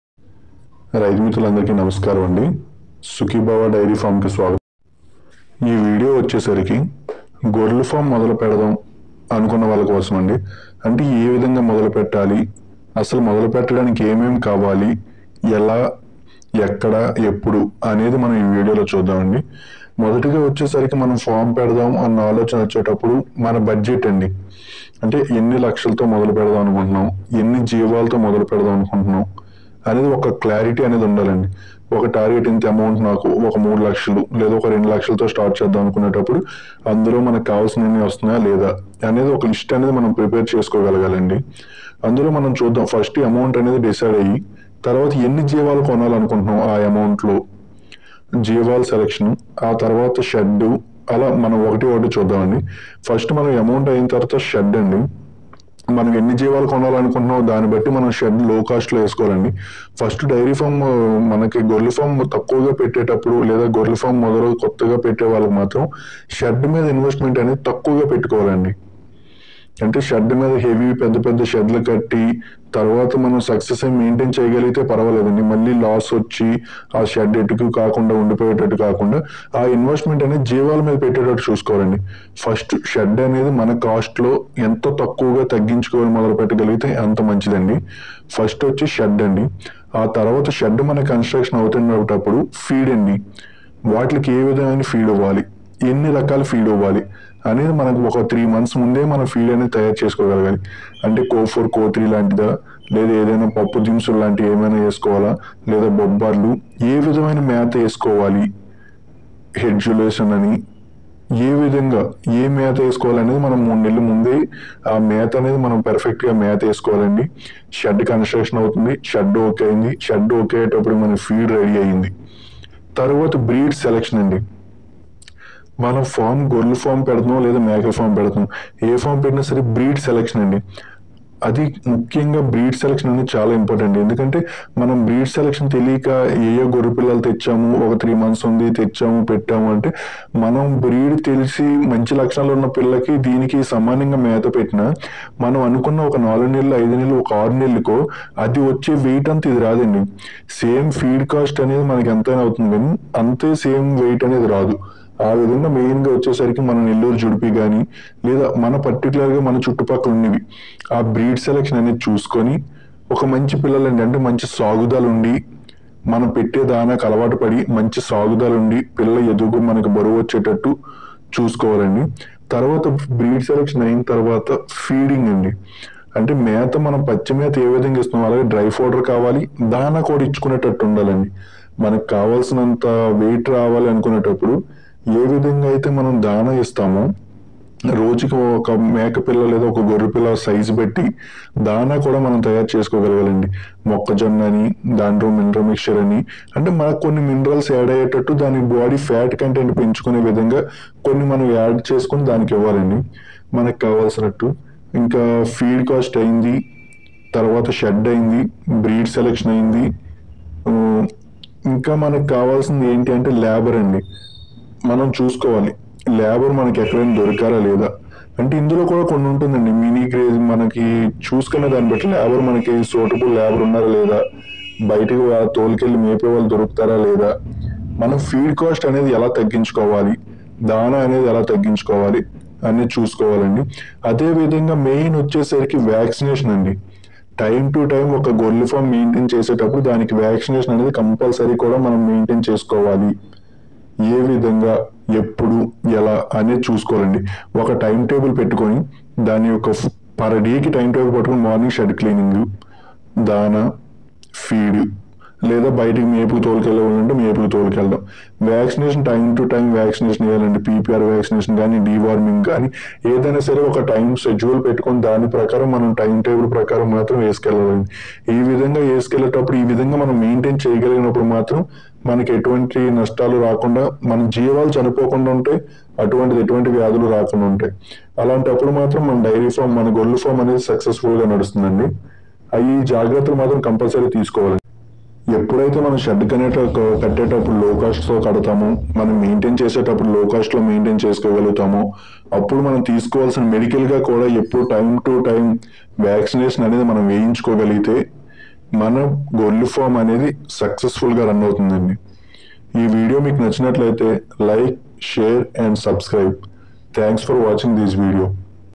గొర్రెలు ఫార్మ్ మొదలుపెట్టడానికి ఏమి కావాలి|ఎలా చేయాలి|how to start sheep farming|sukhibhava dairy.
గొర్రెలు ఫార్మ్ పెట్టాలి అనుకున్నప్పుడు ఏమి కావాలి ,ఎలా చేయాలి.how to start sheep farming,low cost ahead,feeding,grass,labour,breed selection.sukhibhava dairy farm#malleshadla